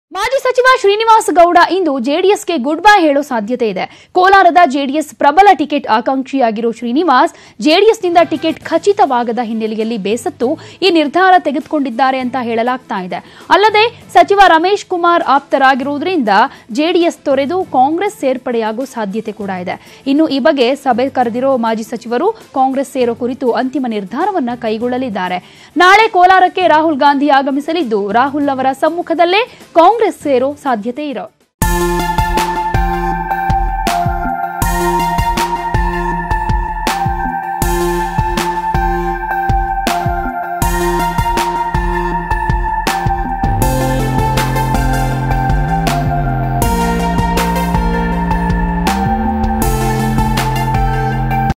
Srinivasa Gowda Quits JDS And Likely To Join Congress ?
Former Minister K Srinivasa Gowda is set to exit JDS to join Congress ahead of Rahul Gandhi's Visit To Kolar tomorrow. He has been denied ticket from Kolar constituency and hence made this big decision to join Congress.